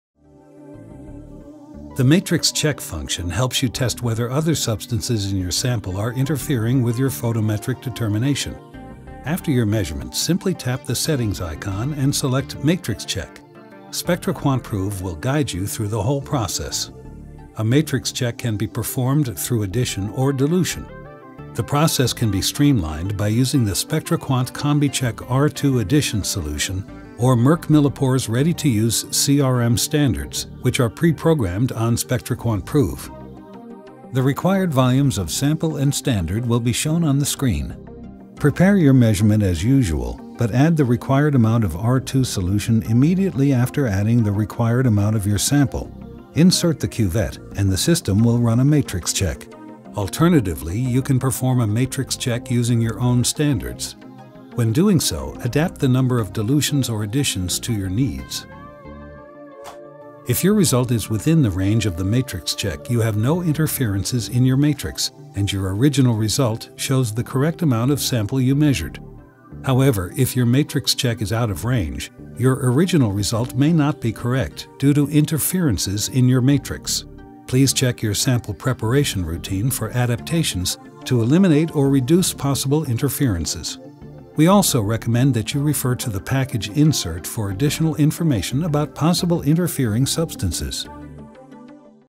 Spectroquant® Prove It just makes your work flow – Guided Matrix Check
Learn more about how to make your water analysis simple, secure and durable with the new Spectroquant® Prove.